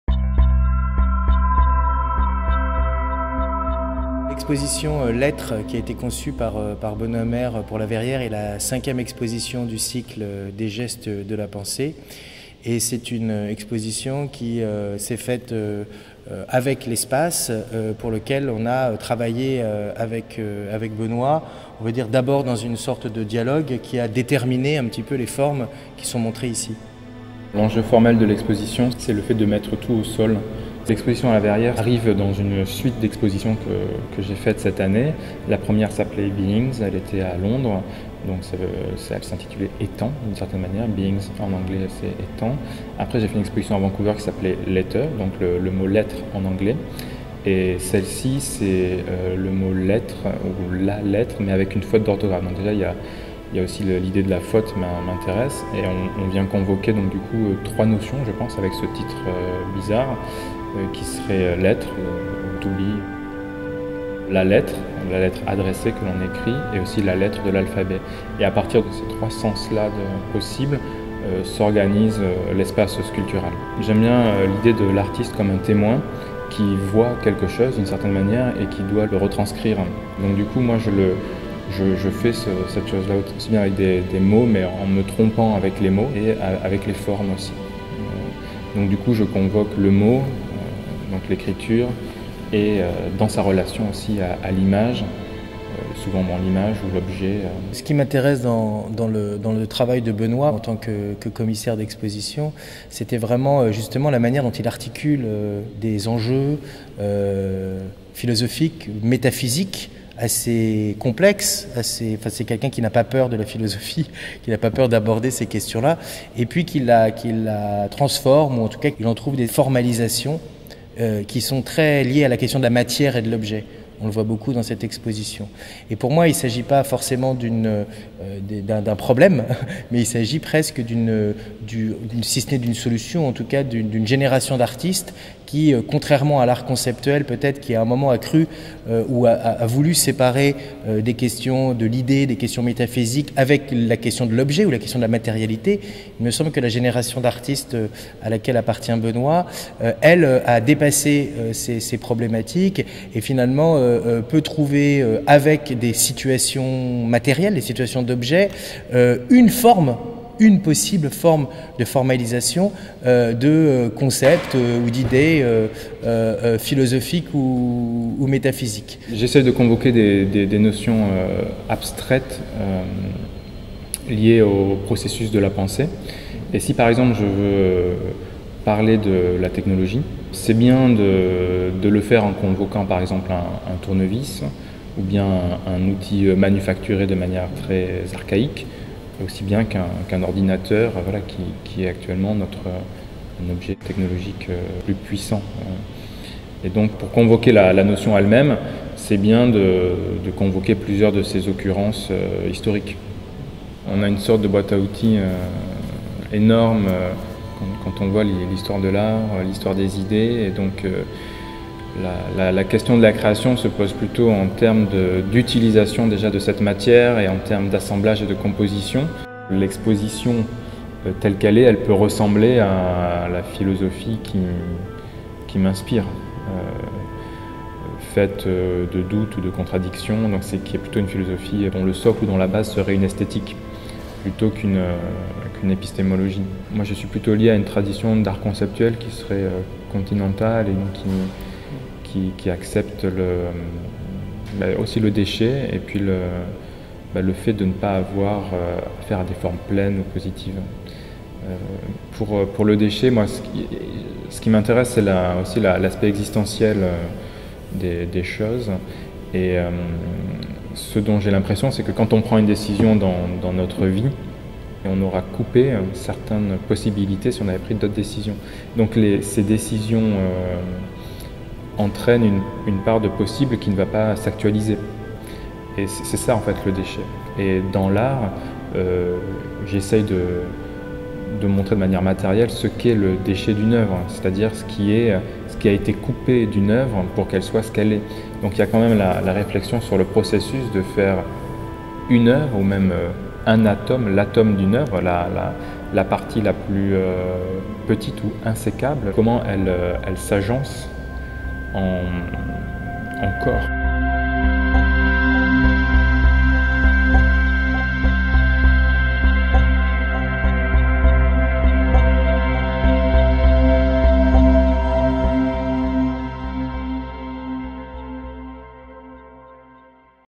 EXHIBITIONS | Benoît Maire at La Verrière
'LETRE is a performance in the present tense, the essence of moments of being, in writing.' The fourth exhibition in the "Gesture, and thought" series at La Verrière presents a minimalist installation by French artist Benoît Maire, marking the latest stage in the philosophical exploration underpinning his work.